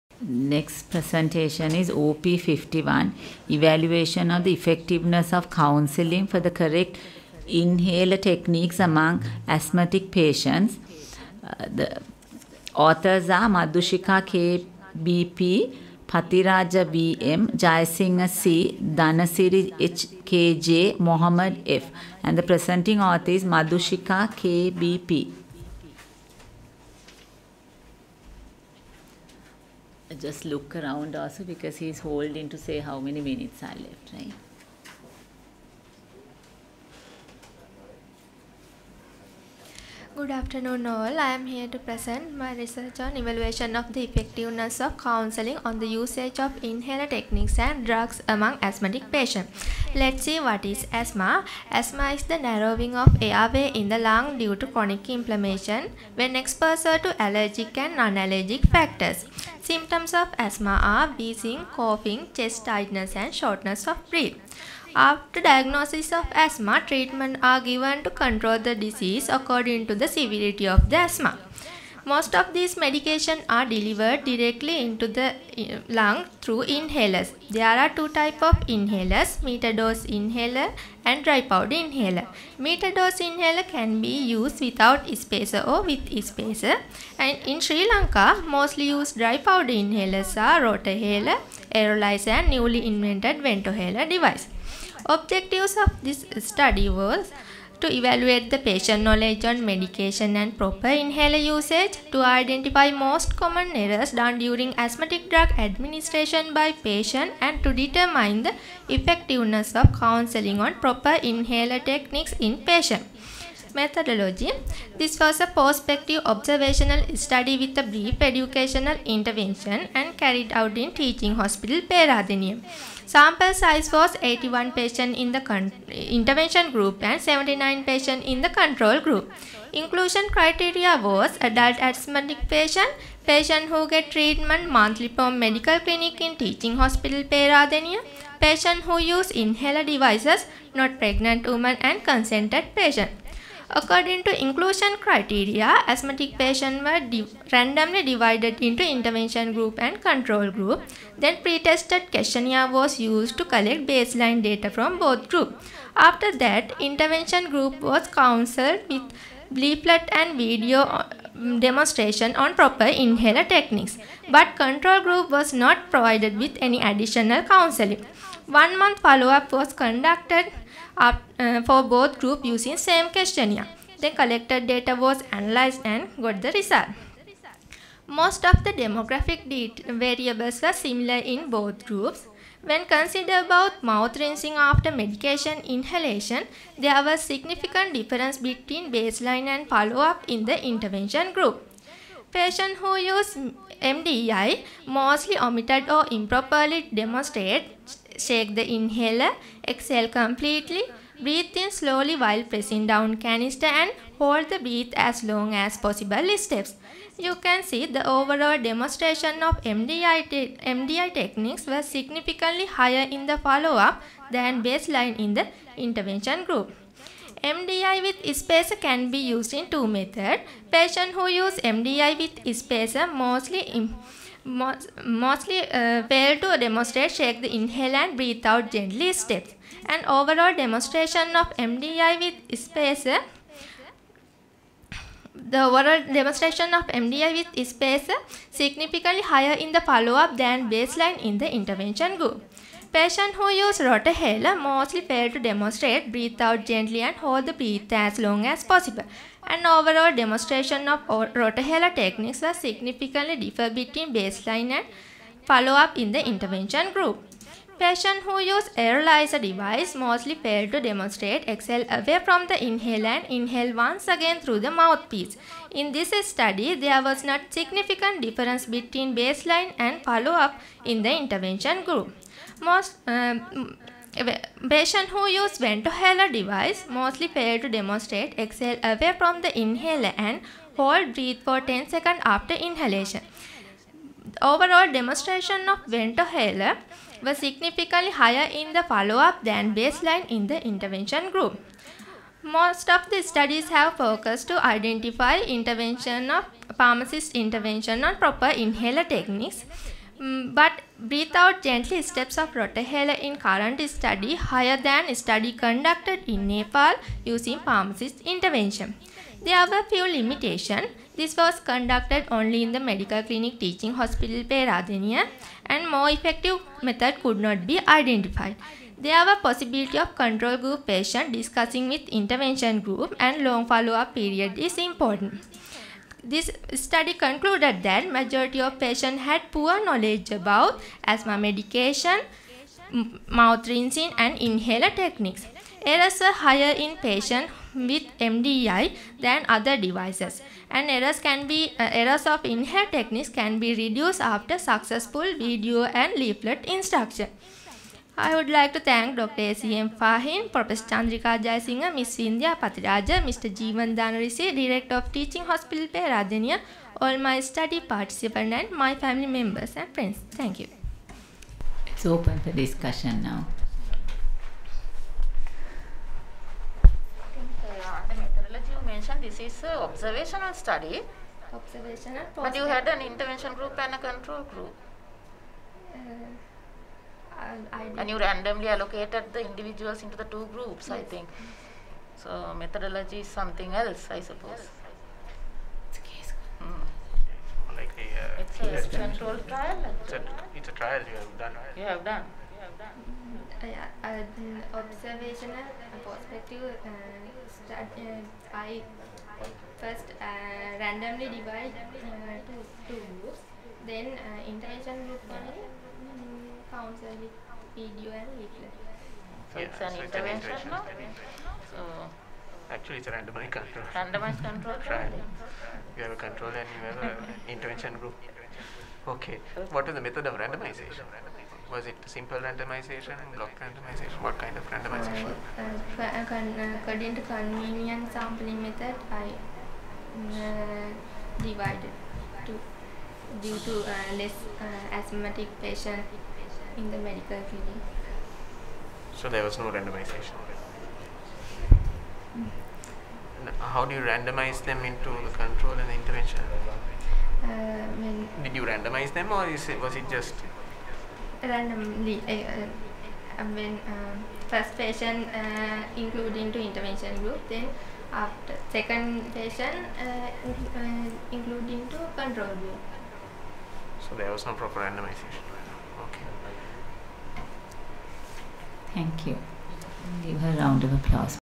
OP 51 Evaluation of the Effectiveness of Counselling for the Correct Inhaler Techniques
EVALUATION OF THE EFFECTIVENESS OF COUNSELLING FOR THE CORRECT INHALER TECHNIQUES AMONG ASTHMATIC PATIENTS
Madushika KBP,  Pathiraja VM, Jayasinghe C,  Dhanarisi HKJ, Mohamed F